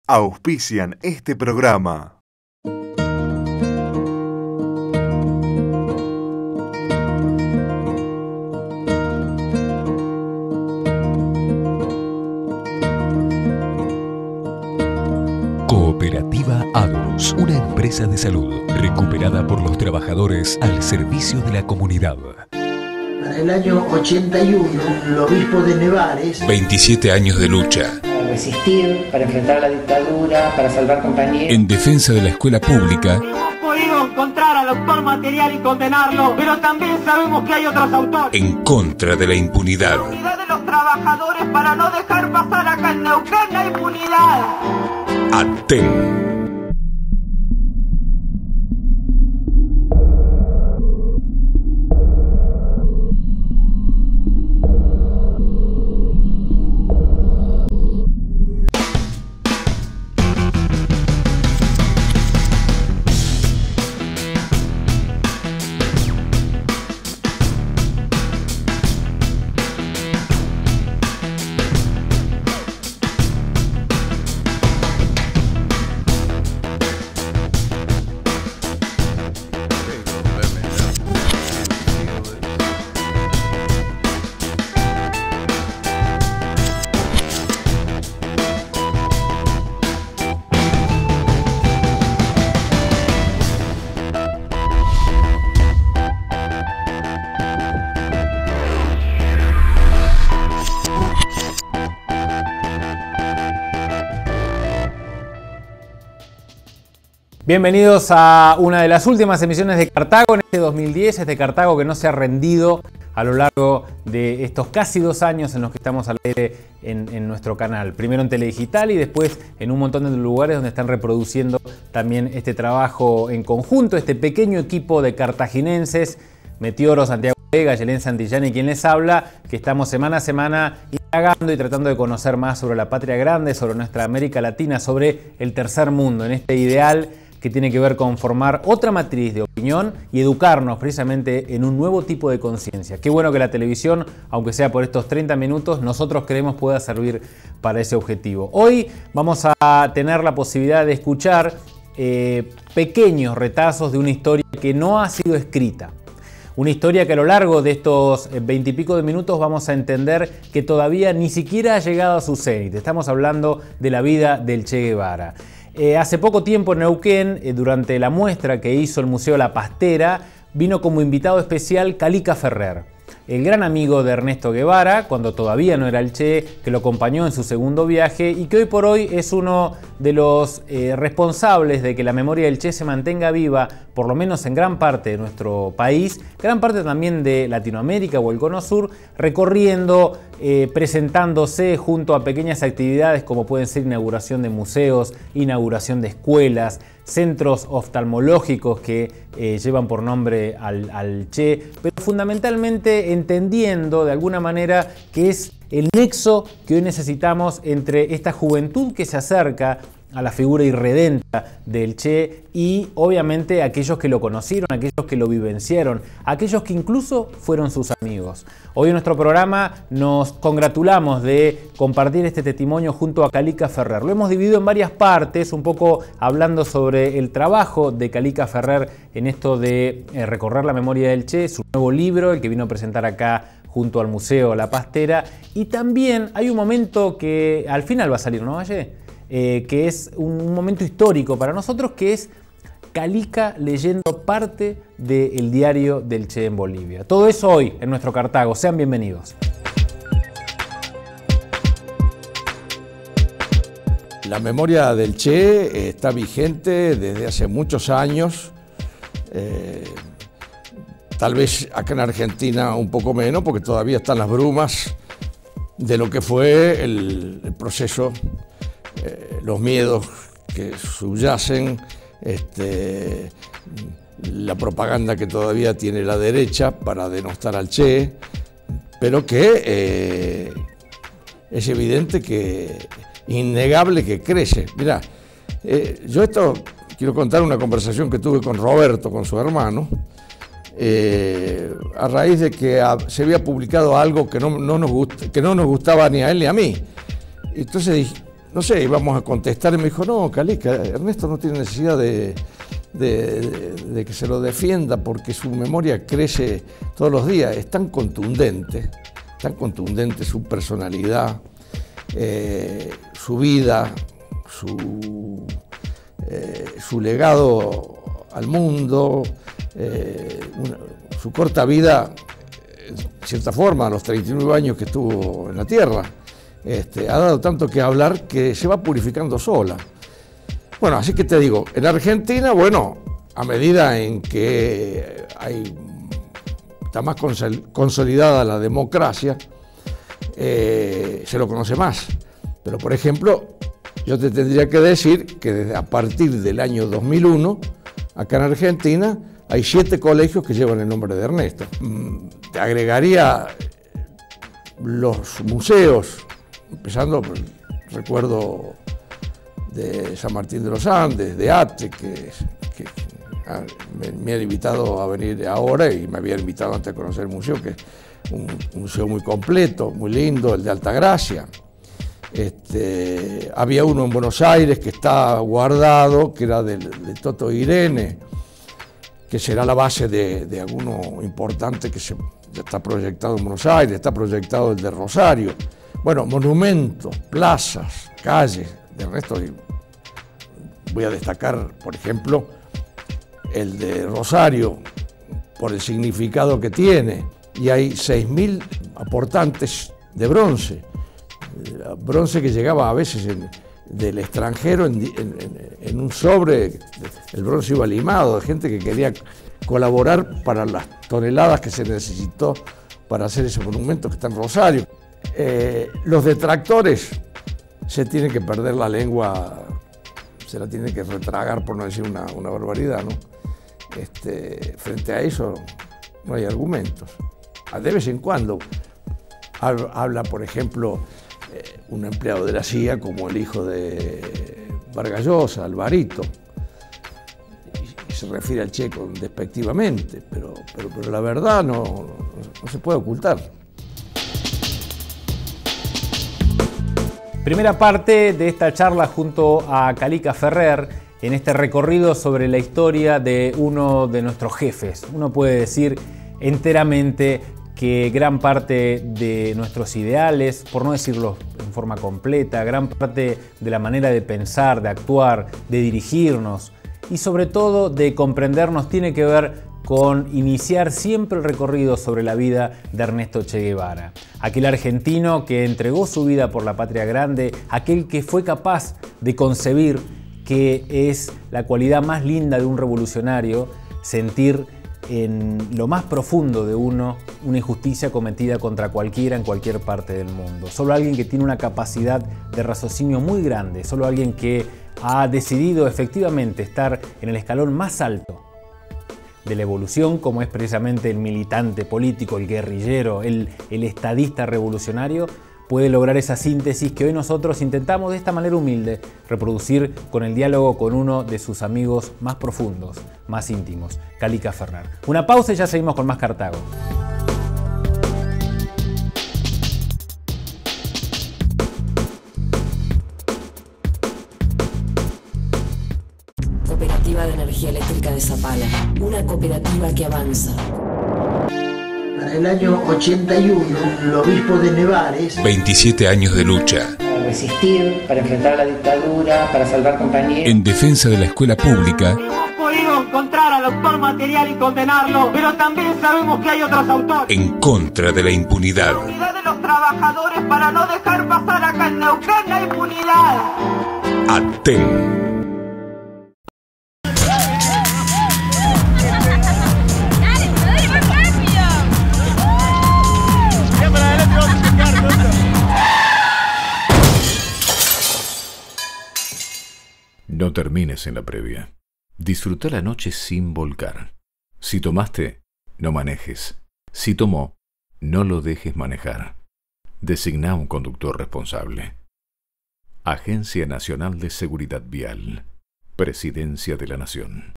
CARTAGO 2010 CALICA FERRER MI AMIGO EL CHE
Primera parte de la entrevista a Calica Ferrer, compañero del Che en su segundo viaje